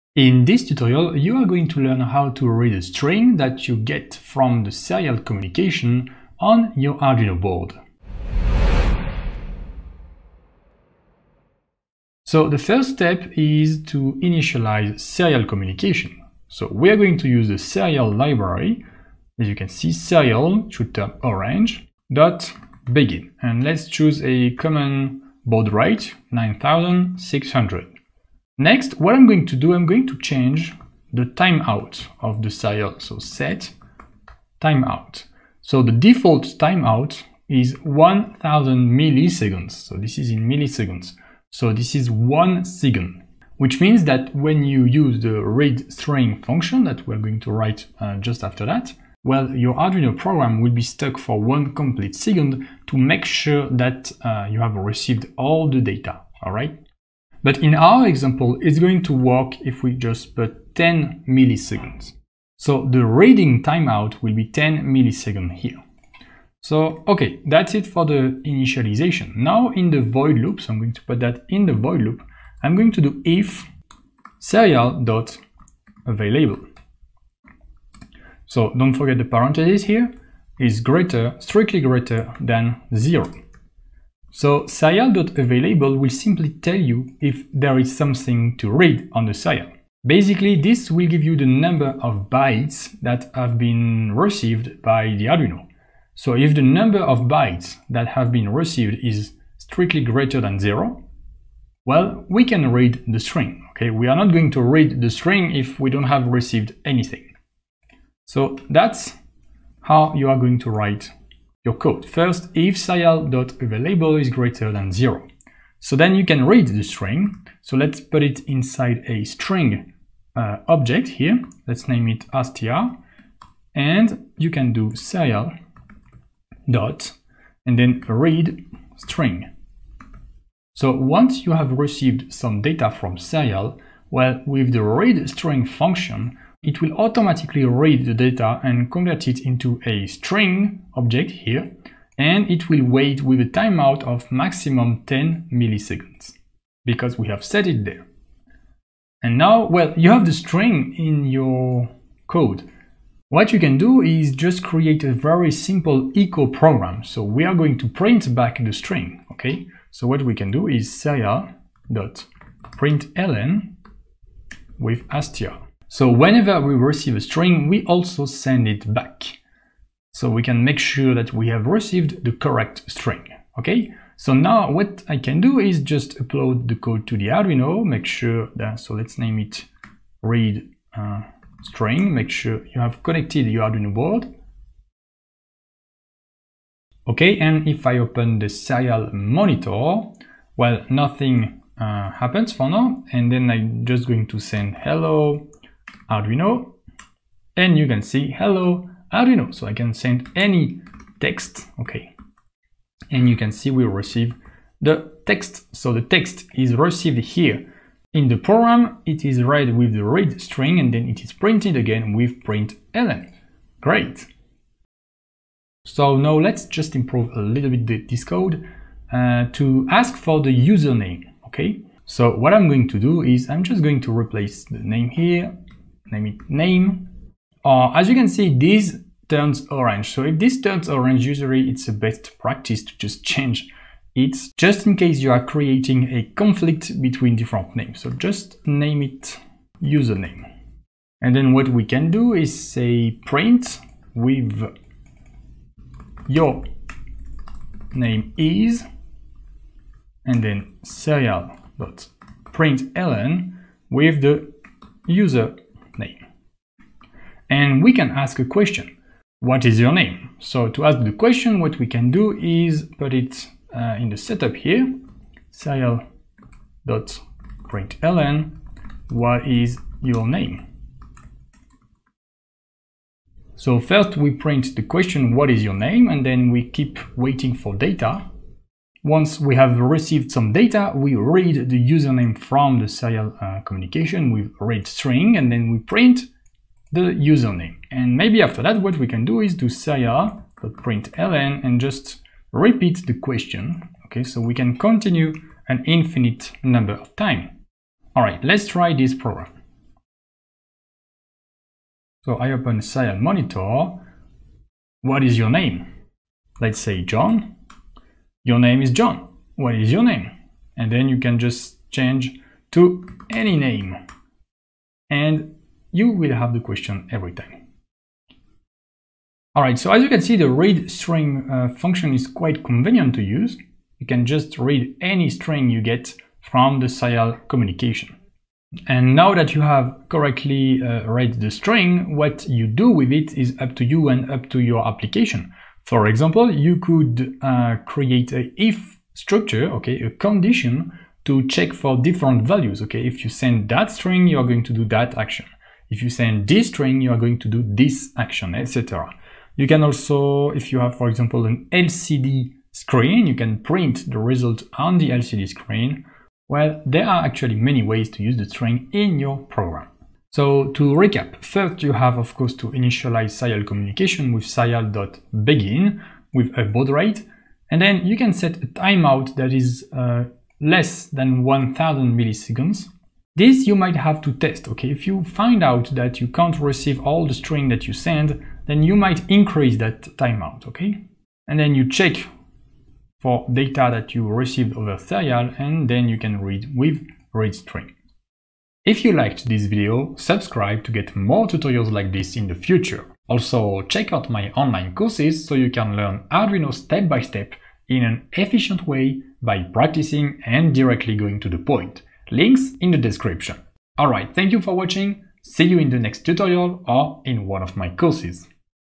Arduino - How to Get a String from Serial with readString()
Learn how to read Arduino String objects directly with the Serial readString() function.

✅ Corresponding Written Tutorial: How to Get a String from Serial with Arduino readString

0:00 Intro
0:10 Initialize Serial and reading timeout
1:05 Write the code with readString()
2:53 Test the readString() function with Serial Monitor
3:35 Ask the user's name
5:18 Best practices
6:28 Outro